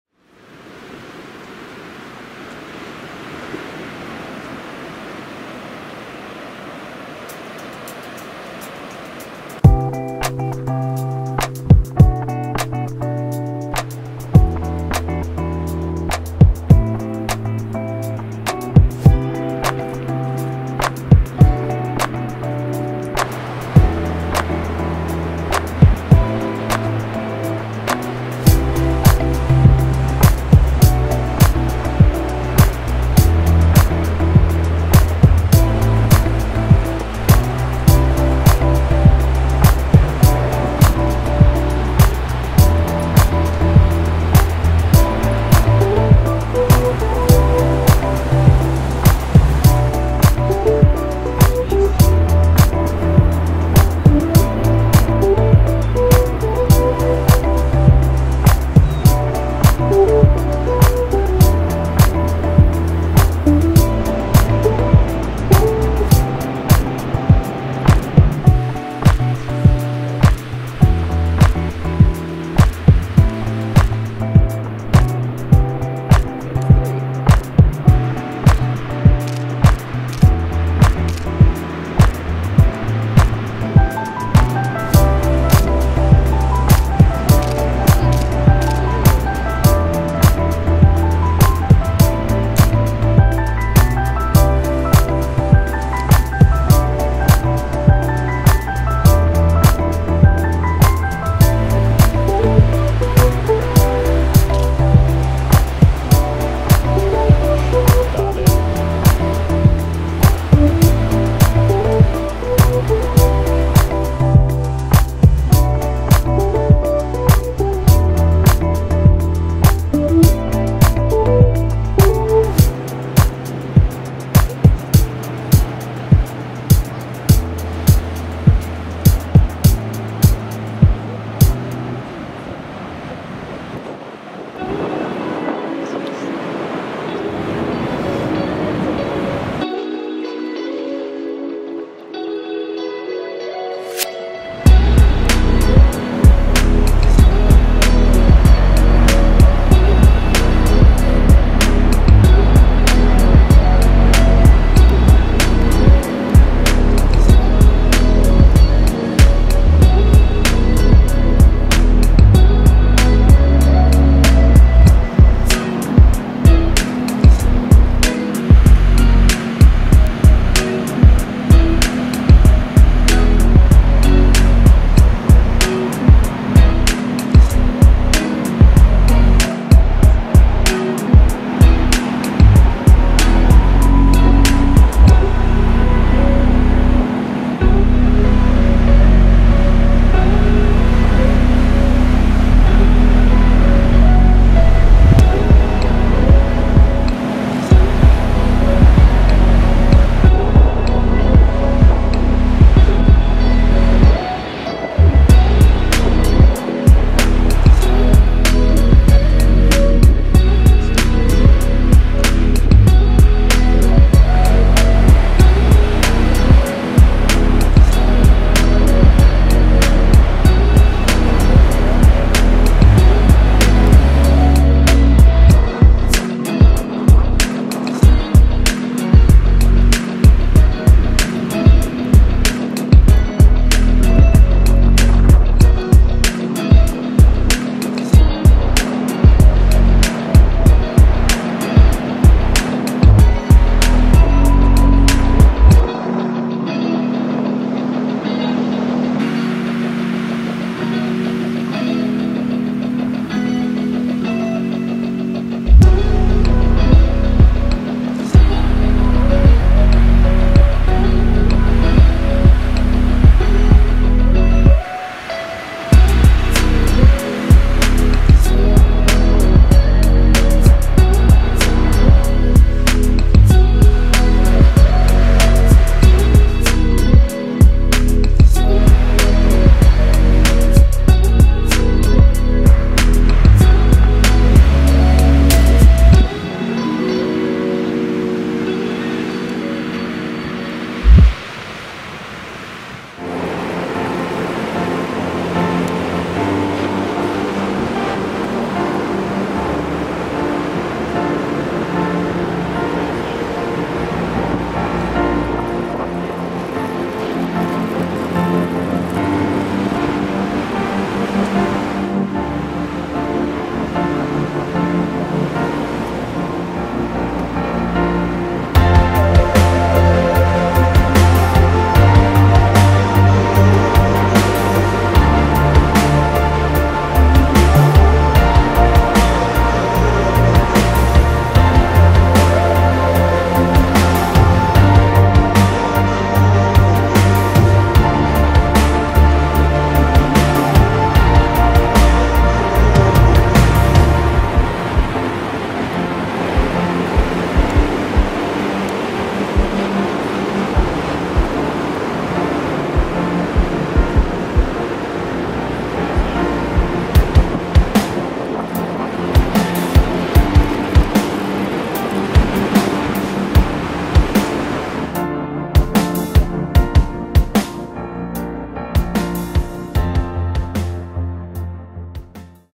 Cape Byron - Dolphin Watch at Byron Bay, New South Wales - 4K
This video is about my trip to Byron Bay back in August 2018. It was easy to spot those happy dolphins playing around the Byron Bay Wategos beach.